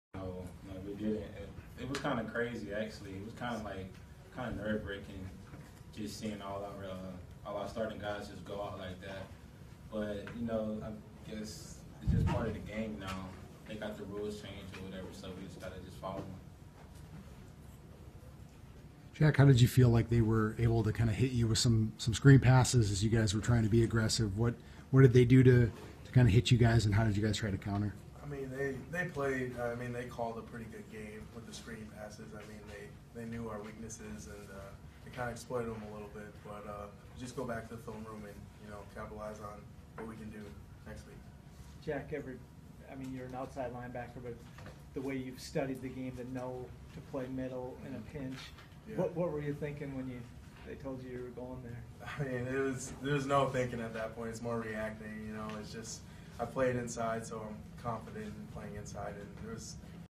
Video: Gophers Damarius Travis and Jack Lynn breakdown 30-23 win over Oregon State.
By: Andy Greder - Writer for @PioneerPress
Published on: September 2, 2016